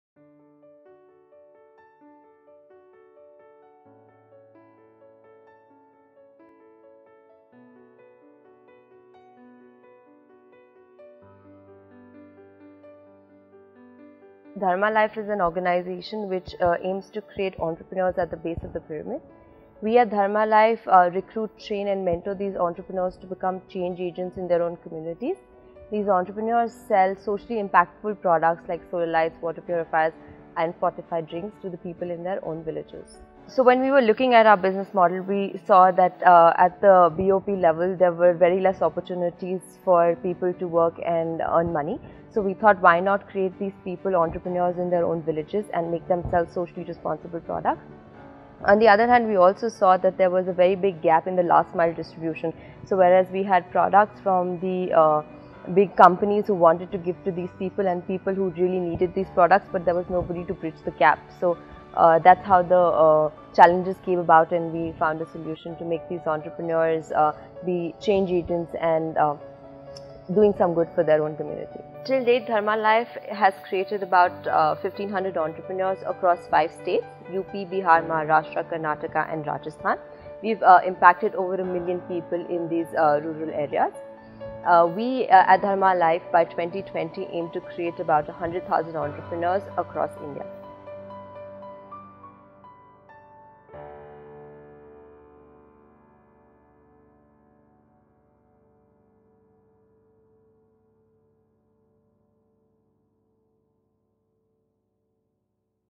Project Dharma - Sankalp Enterprise 2014
Dharma Life aims to provide individuals from rural areas with opportunities to create a livelihood for themselves within their local community, thereby reducing rural to urban migration and rural underemployment. Through rural entrepreneurs, who are provided with a portfolio of socially impactful products such as solar lighting, improved cookstoves, water purifiers, nutritional drinks, and basic hygiene products, the village-based consumers also gain access to products that improves their quality of life.
While their focus remains on creating entrepreneurs and catering to the needs of BoP customers, they have been able to create a highly scalable ecosystem that combines the best in class corporate practices with social impact. By 2020, they aim to recruit, train and mentor more than 100,000 entrepreneurs, which will help improve the lives of more than 50 million people. Till date they have supported 5,900+ village level enterprises and improved the livelihood of 960,000 individuals